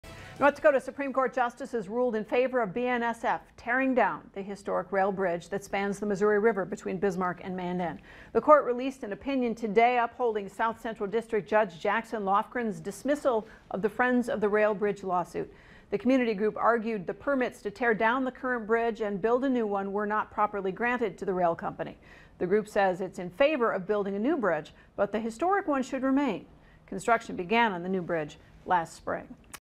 Supreme Court agrees with BNSF in bridge dispute
The North Dakota Supreme Court ruled in favor of BNSF tearing down the historic rail bridge that spans over the Missouri River between Bismarck and Mandan.